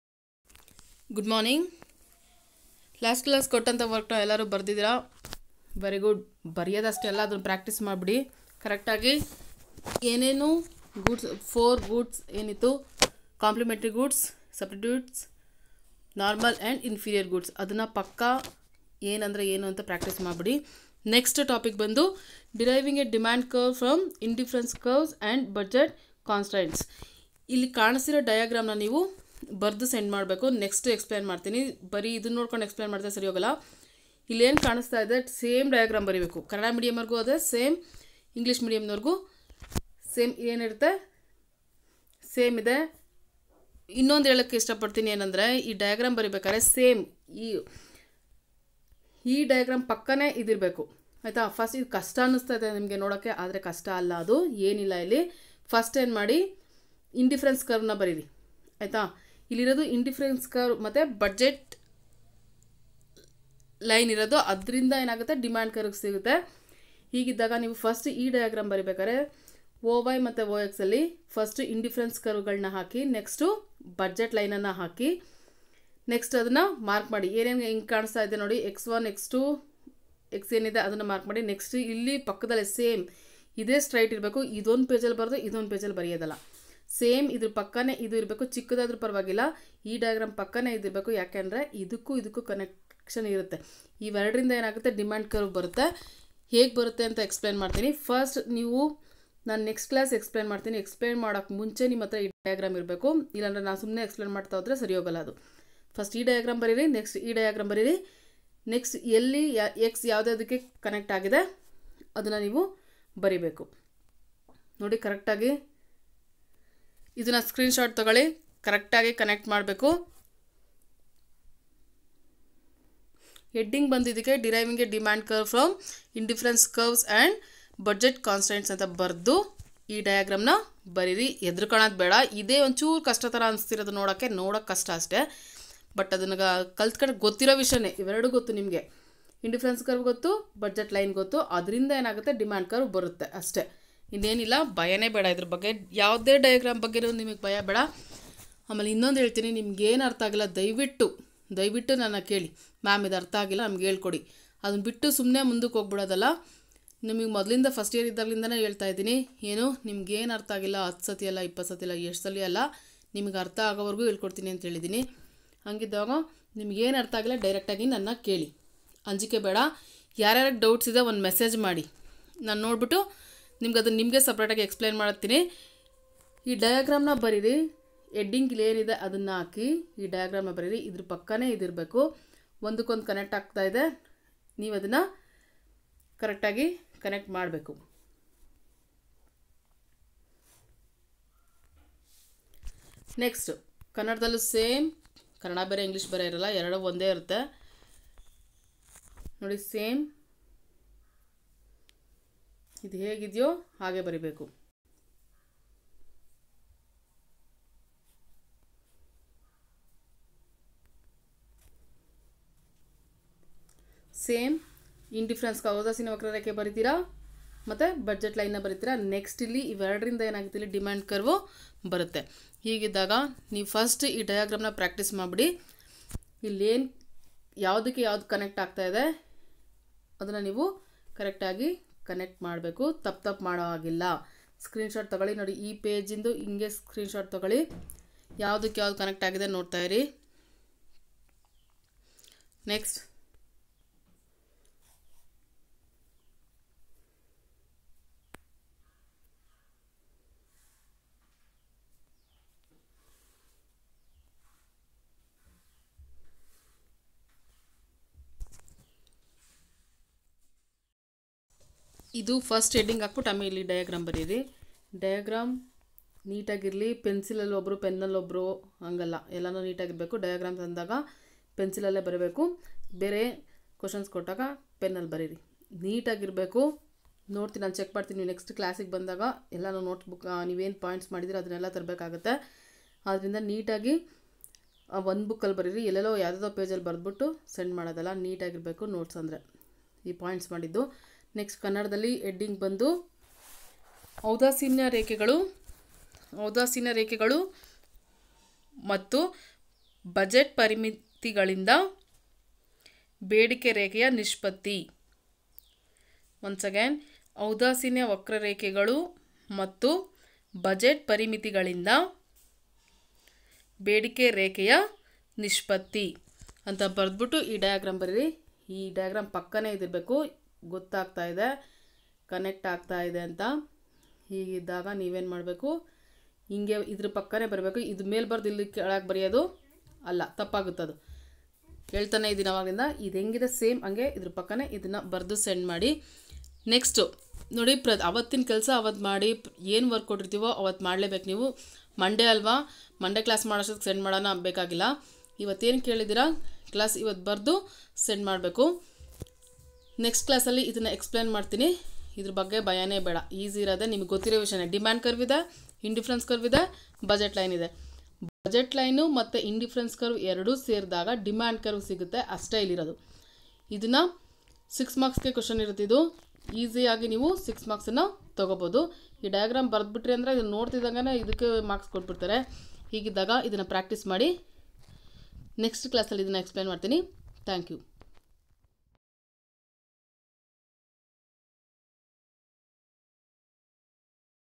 34. Economics class by jalaja N
Deriving a demand curv from indifference curves and budget constraints